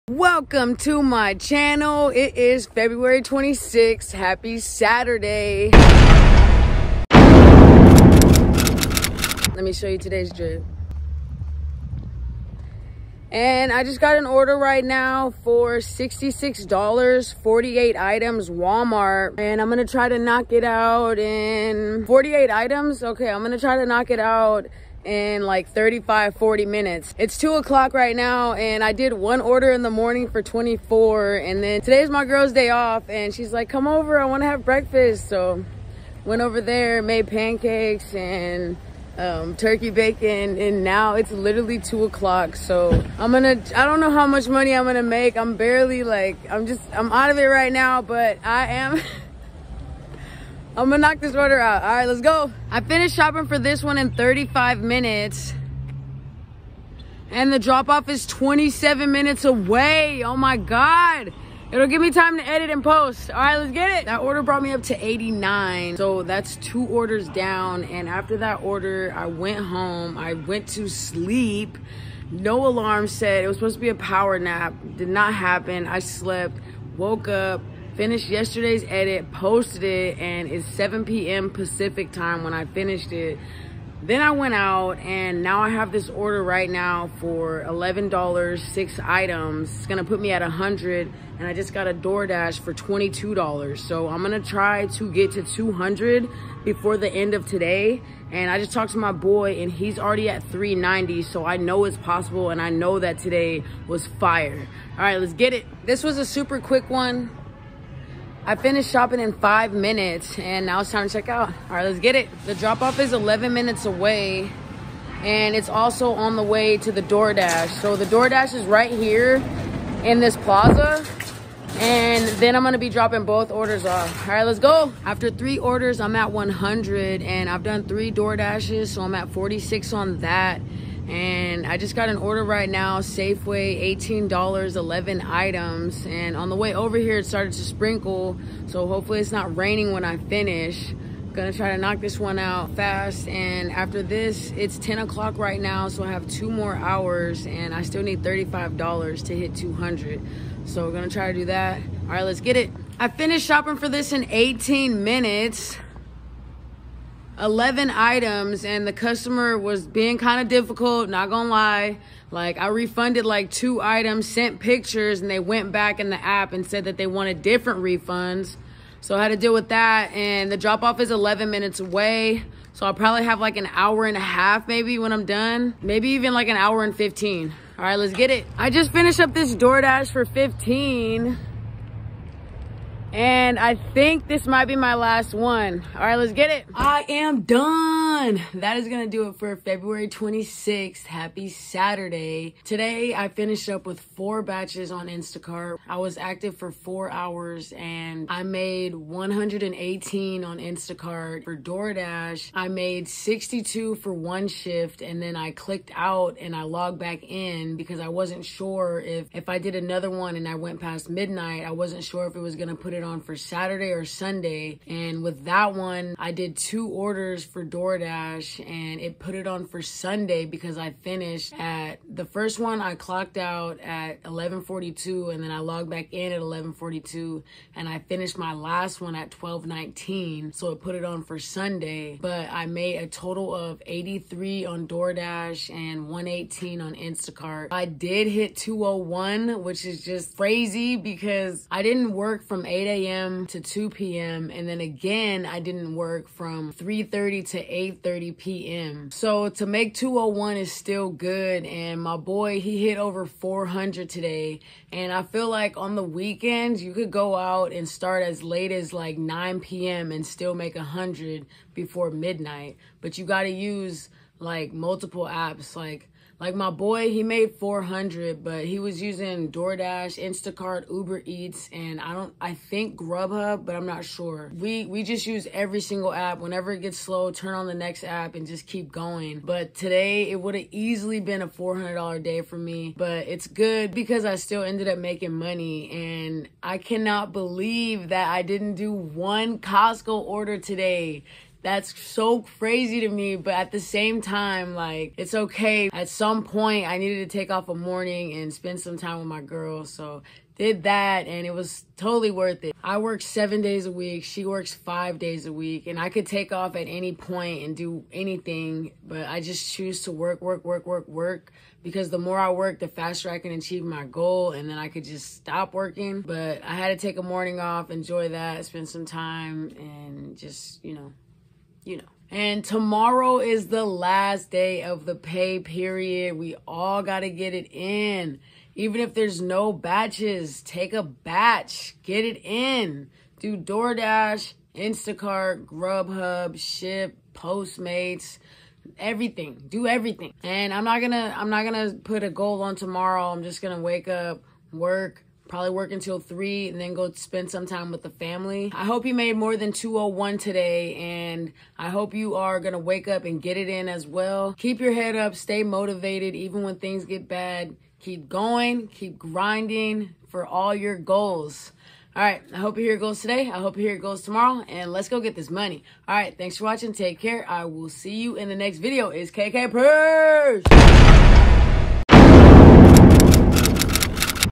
NO COSTCO TODAY With a Late Start - Here is the Outcome February 26 vlog
Young KK

Today I started work at 7am and somehow did not shop at Costco. I did not go to the drop because I went to my girlfriends house for breakfast. When I left it was 2pm and I did one batch and went to sleep. When I woke up I worked until midnight, how much did I make today?